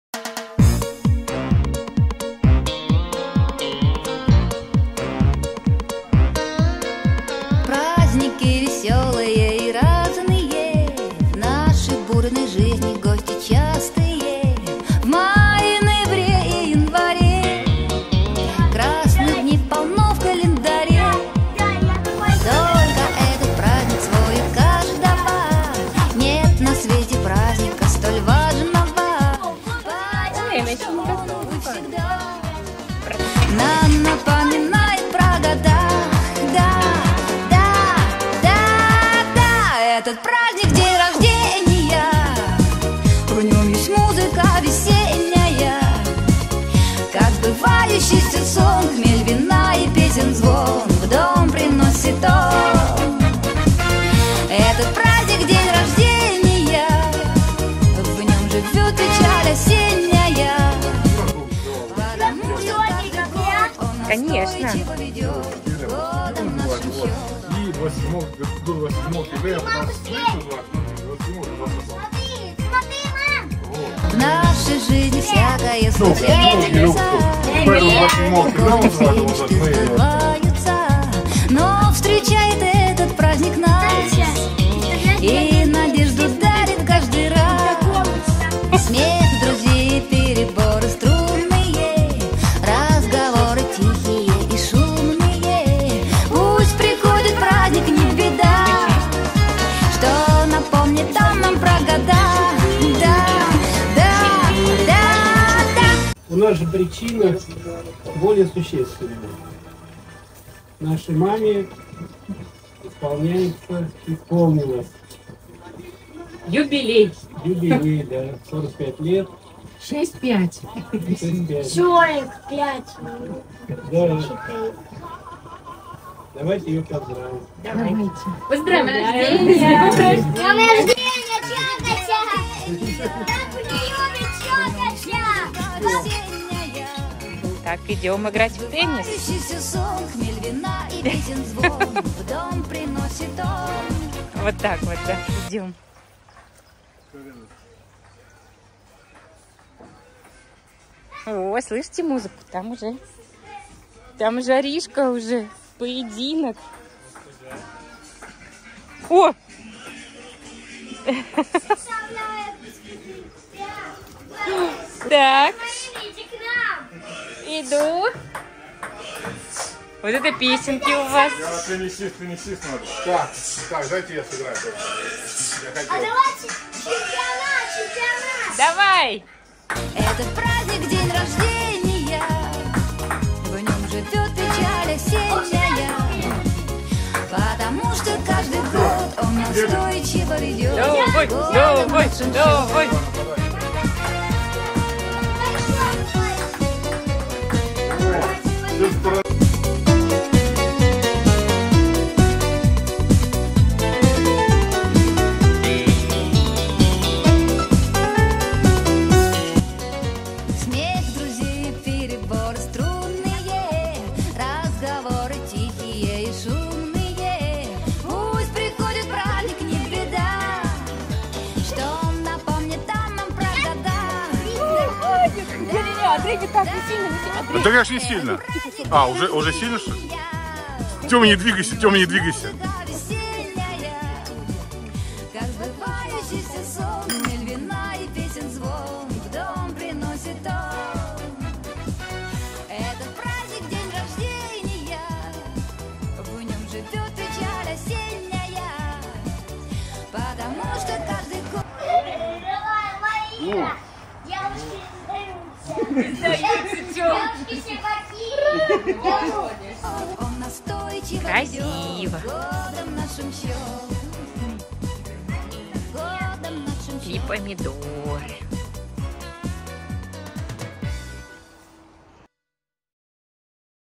ОТДЫХАЕМ НА ДАЧЕ, ОТМЕЧАЕМ ДЕНЬ РОЖДЕНИЯ МАМЫ. ВЛОГ
Всем привет!
7 июля у мамы был день рождения, собрались всей семьей, решила запечатлеть этот момент и смонтировать душевный видеоролик.
Если вы тоже проникнитесь атмосферой, буду только рада!